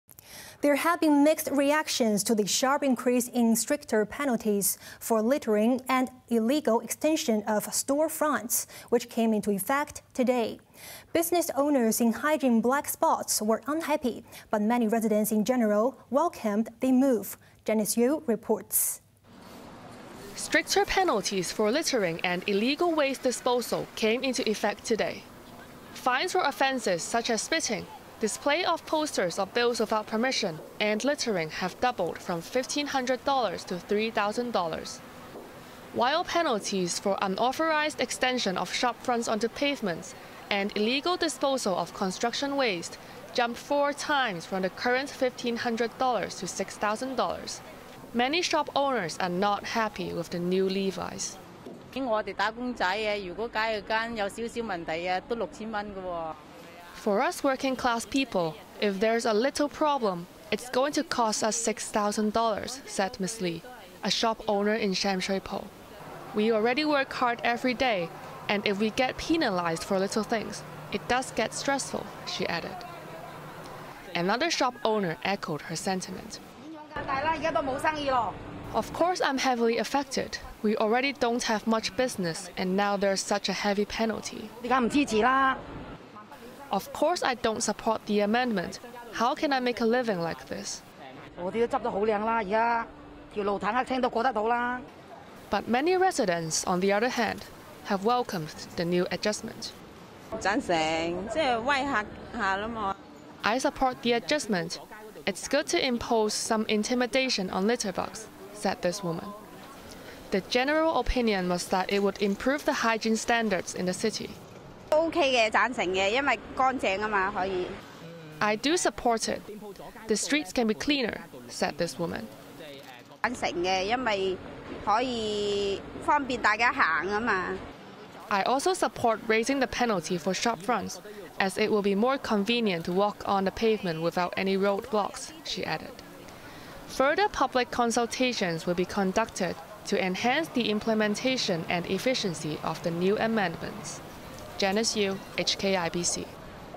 Stricter Penalties on Littering and Illegal Extensions | HKIBC News
There have been mixed reactions to the sharp increase in stricter penalties for littering and illegal extension of storefronts which came into effect today.

Business owners in hygiene blackspots were unhappy but many residents in general welcomed the move.